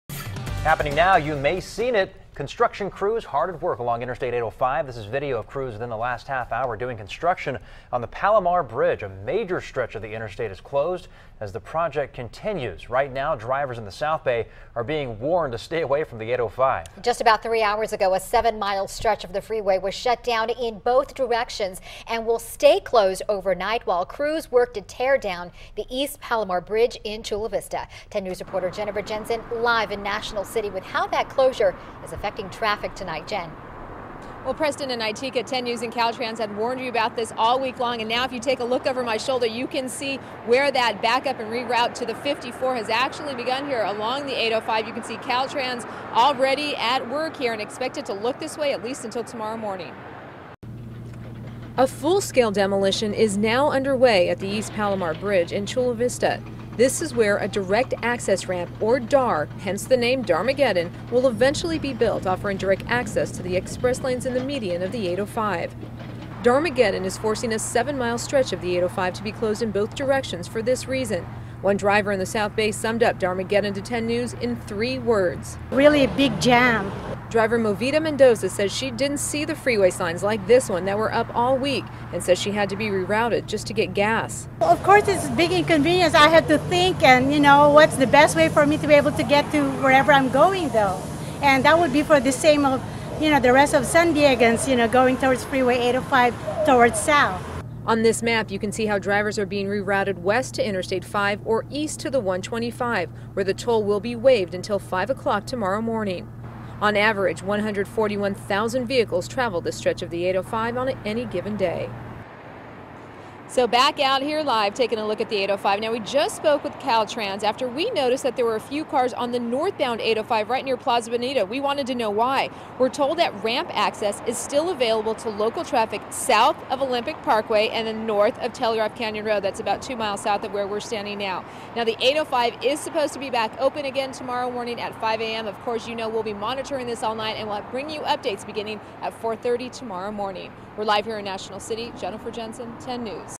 7-mile stretch of Interstate 805 in South Bay closed for Caltrans' 'DARmageddon' project
A seven-mile stretch of Interstate 805 in the South Bay was shut down in both directions on Sunday for Caltrans' "DARmageddon" project.